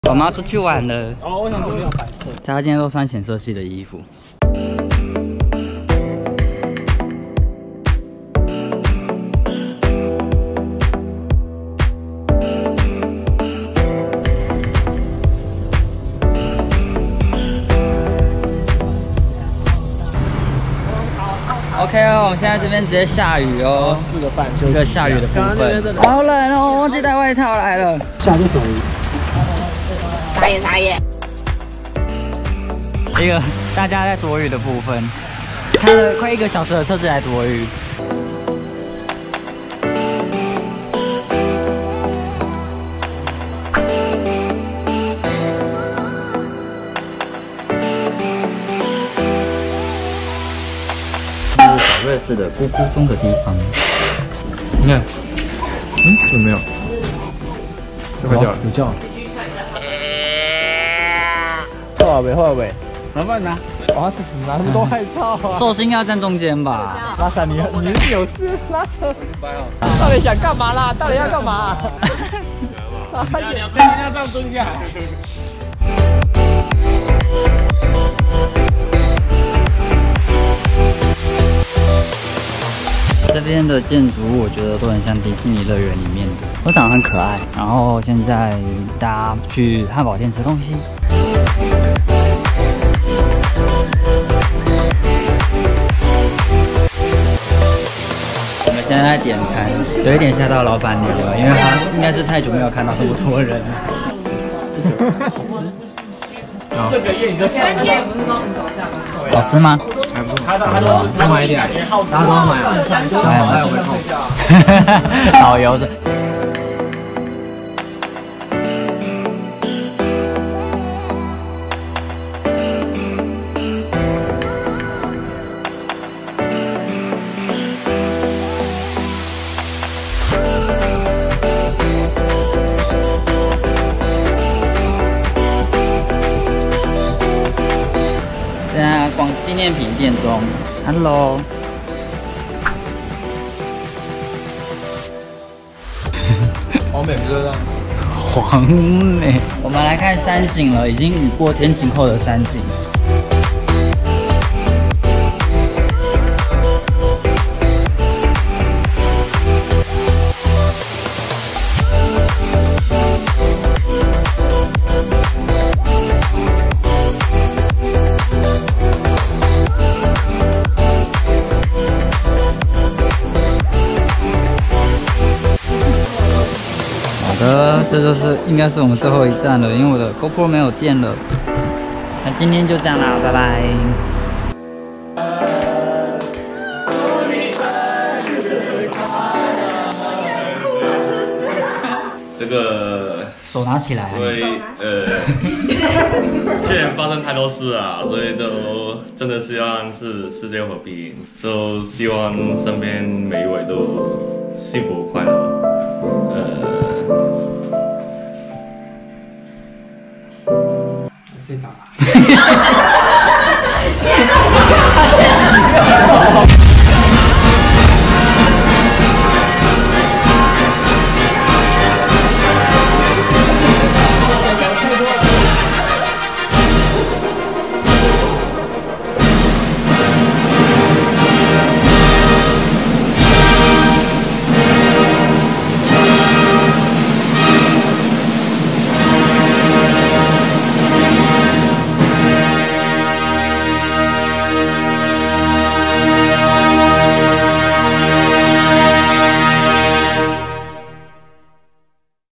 昆士蘭小瑞士 Montville｜Ethan Vlog澳洲打工生活#32
這一天大家一起出門，享受久違的戶外活動
可惜天氣不太好，但雨過天晴後的山景很美
這一次影片後面還是有彩蛋喔哈哈哈